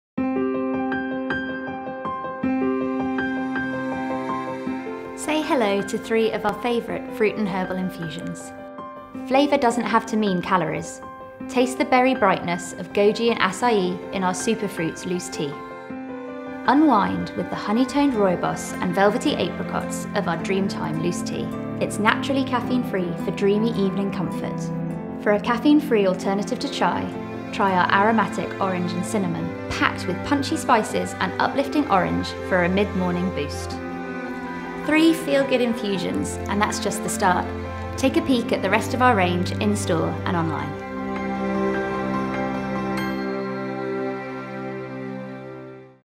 Whittard | What’s in our Fruit & Herbal Tea?
Whether you fancy a mid-morning boost or an evening wind-down, our feel-good fruit and herbal infusions will see you right. Watch as our tea expert Bethan introduces a few favourites from the range.